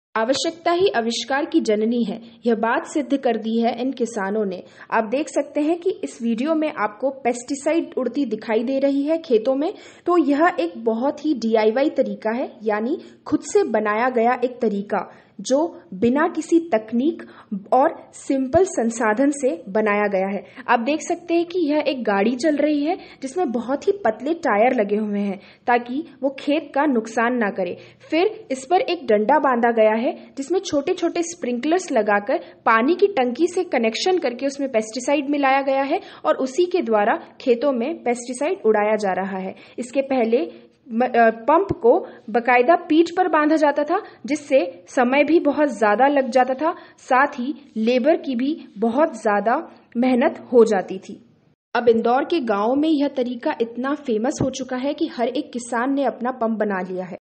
जुगाड़ हो तो ऐसा… किसान ने बनाई पेस्टिसाइड स्प्रिंकलर मशीन, अब चुटकी में होगा घंटों का काम, जानें की
इन दिनों इंदौर से जुड़े गांव में एक बहुत ही ट्रेंडिंग तकनीक किसानों के बीच प्रसिद्ध हो चुकी है, और यह कोई बड़ी कंपनी या ब्रांड द्वारा बनाई गई मशीन नहीं बल्कि किसानों के अपने ही जुगाड़ का परिणाम है.